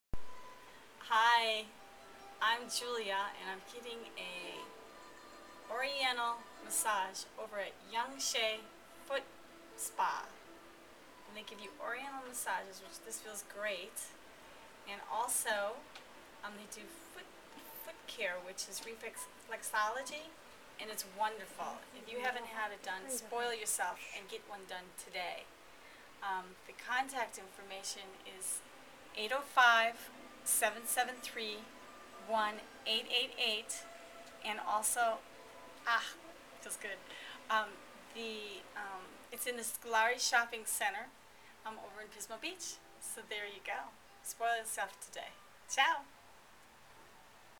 Yang Sheng Foot Spa
Oriental massage and foot spa in the central coast California.. Feels great.
for more fun things to do on the central coast California pismo beach, shell beach, avila beach.